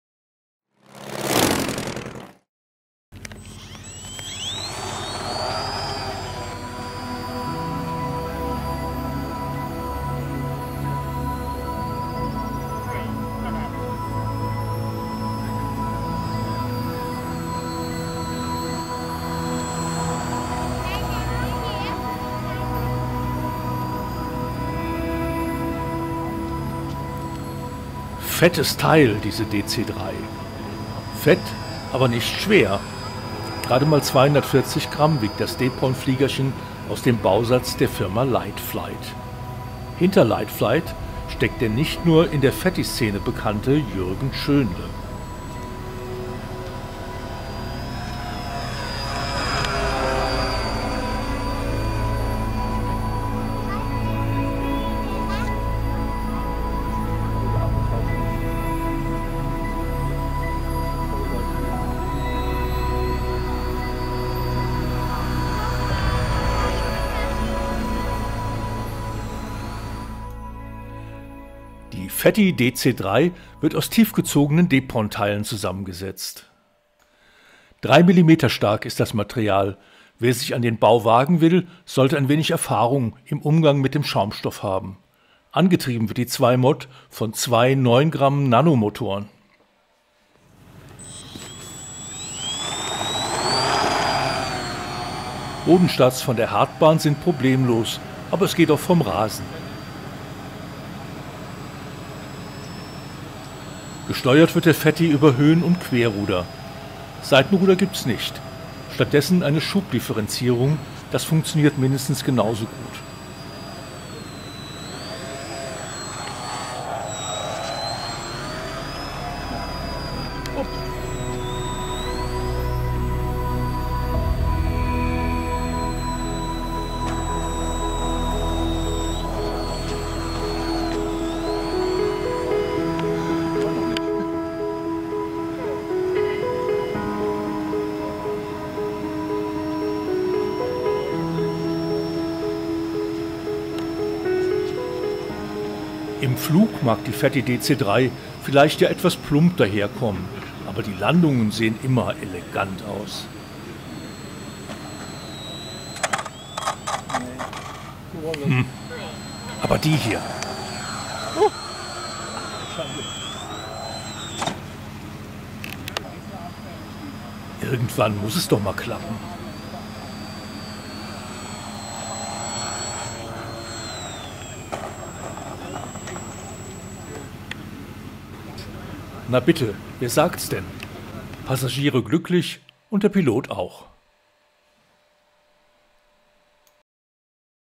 DC-3 Fatty von LightFlight - Video zum Testbericht in FlugModell 4+5/2024 von Lutz Näkel
Gestauchter Rumpf, Stummelflügel und Cartoon-artige Proportionen, das zeichnet Fatties aus. Ob die DC-3 Fatty von LightFlight auch gut fliegt, und nicht nur krass gut aussieht, zeigt Lutz Näkel in seinem Video. Einen sehr ausführlichen Beitrag mit vielen Tipps und Tricks zum Bau des Depron-Modells gibt es in FlugModell 4+5/2024. Schaut da mal rein!